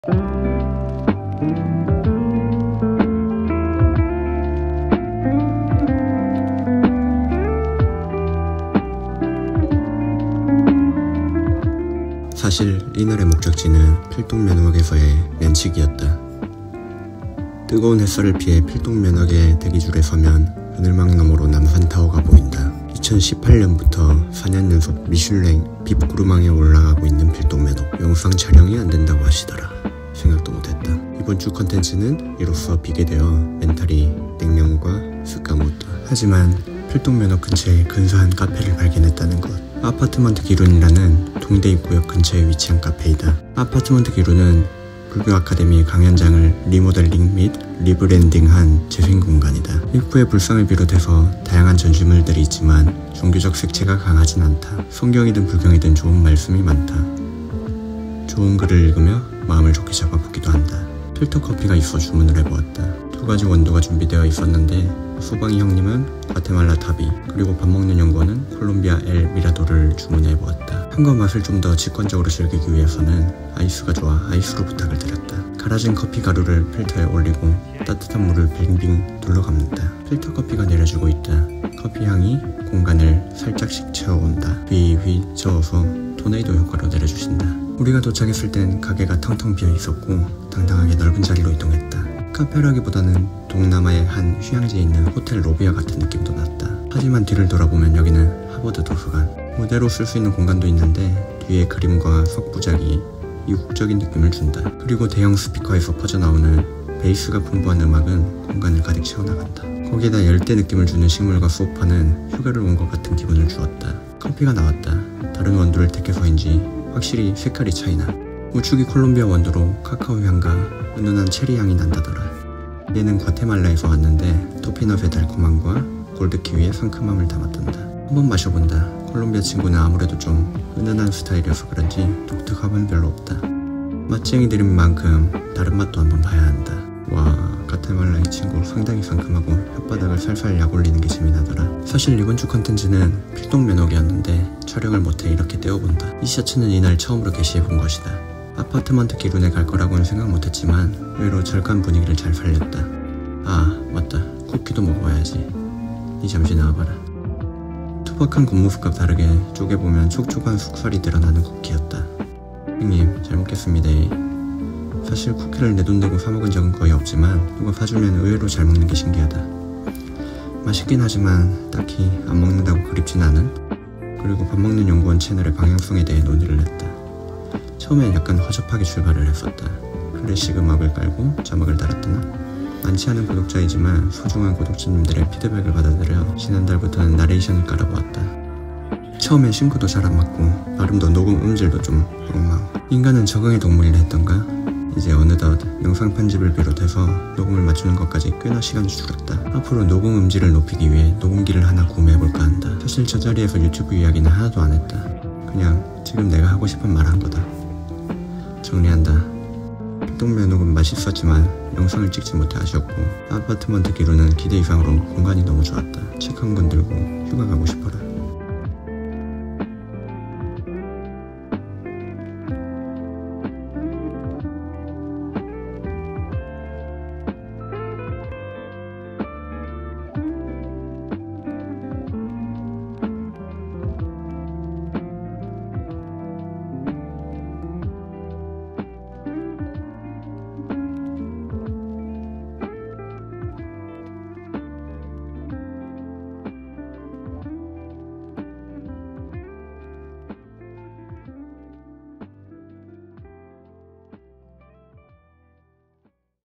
시원한 여름의 시작 냉면 한 그릇과 아이스 드립커피 '아파트먼트 기룬'
스님이 젤 좋아하는 나무아샷츄~

© Music produced by Room7, distribute and publishing by Room7.